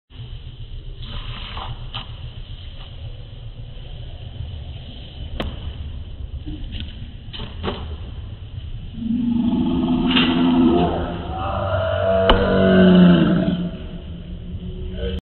Bboy jumpshot
Bboy Butcher lands upside down jumpshot on one hand